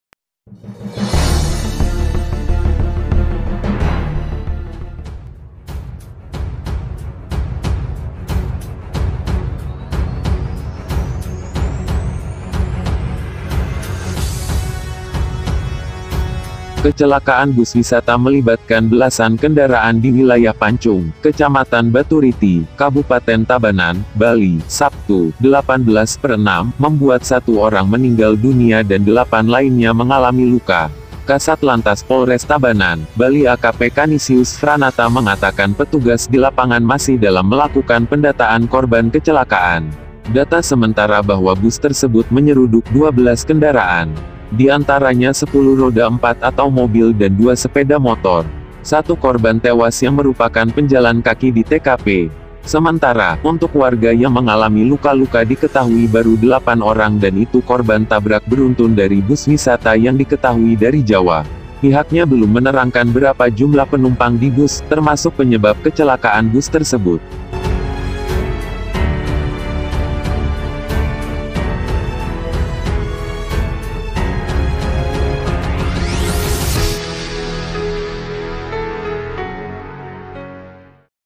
BUS SERUDUK 10 MOBIL , 2 MOTOR 1 DI TABANAN BALI
Kecelakaan bus wisata melibatkan belasan kendaraan di wilayah Pancung, Kecamatan Baturiti, Kabupaten Tabanan, Bali, Sabtu (18/6) membuat satu orang meninggal dunia dan delapan lainnya mengalami luka.

Kasat Lantas Polres Tabanan, Bali AKP Kanisius Franata mengatakan petugas di lapangan masih dalam melakukan pendataan korban kecelakaan.

Data sementara bahwa bus tersebut menyeruduk 12 kendaraan. Di antaranya 10 roda empat atau mobil dan 2 sepeda motor. Satu korban tewas yang merupakan penjalan kaki di TKP.

Sementara, untuk warga yang mengalami luka-luka diketahui baru 8 orang dan itu korban tabrak beruntun dari bus wisata yang diketahui dari Jawa. Pihaknya belum menerangkan berapa jumlah penumpang di bus, termasuk penyebab kecelakaan bus tersebut.